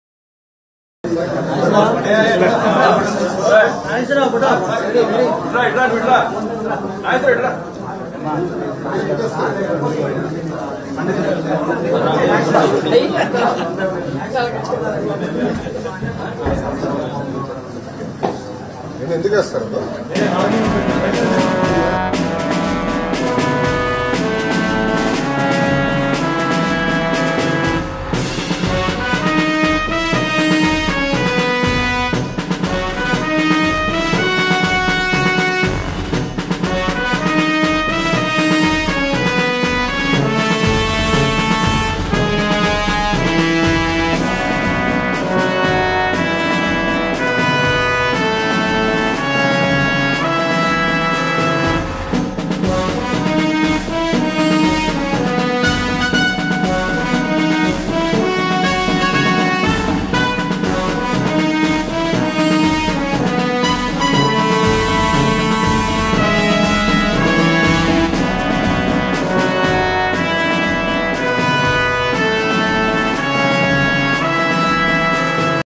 Nara Lokesh Seriously Worning To Staff | ఇలాంటి పనులు ఇంకా ఎప్పుడు చేయకండి
సచివాలయం నాలుగో బ్లాక్ లోని ఛాంబర్ లో బాధ్యతలు స్వీకరించిన నారా లోకేష్. తన కుర్చీకి ఎలాంటి ఆర్భాటాలు వద్దని, తన కుర్చీకి కట్టిన టవల్ను స్వయంగా తీసివేసిన మంత్రి లోకేష్...